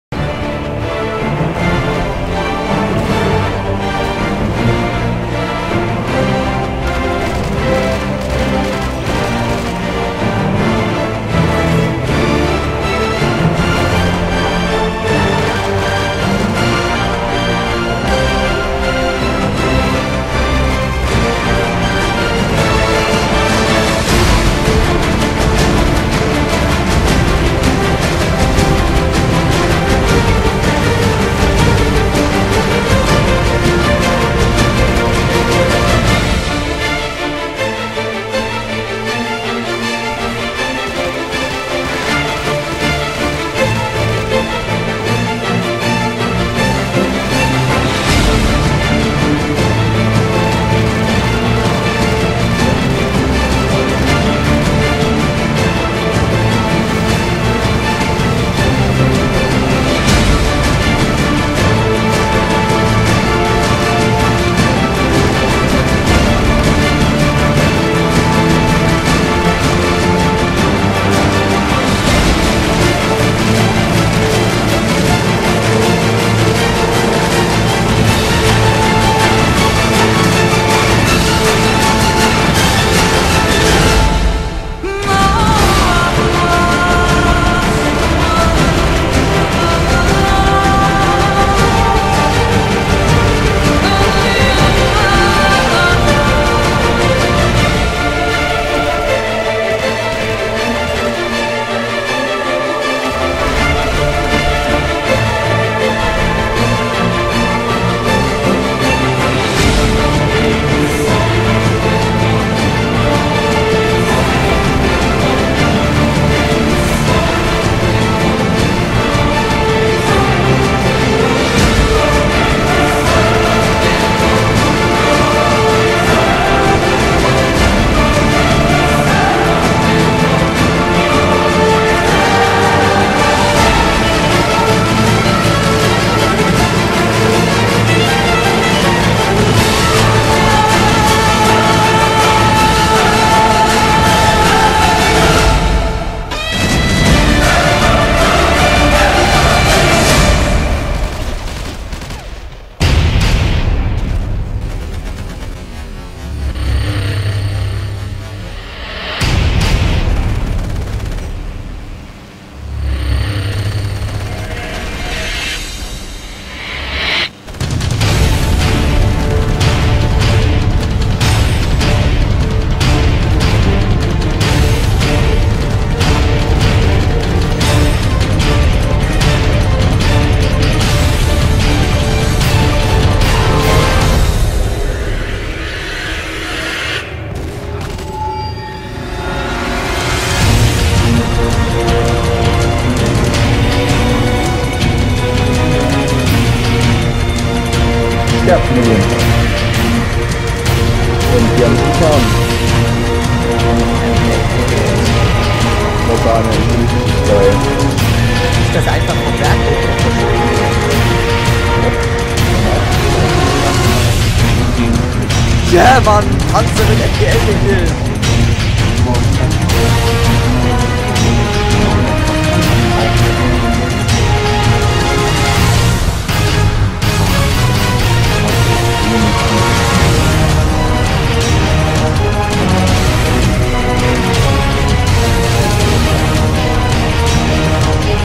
A Hero, born to defeat em - XXWarwick Montage
Soweit ich mich erinnere, schulde ich euch noch ne Montage zum 50 abo-special^^
Ich hoffe, ich seid zufrieden mit dieser hier.
Nicht vergessen sich des ganze auf HD anzugucken. ;-)